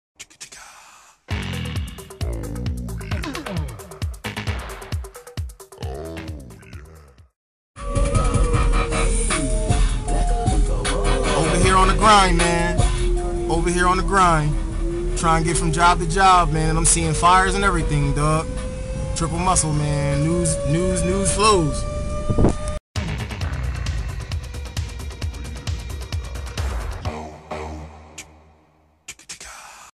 Truck on Fire!!! It's Getting Hot In Here
A fire adventure with Larry on the job!!!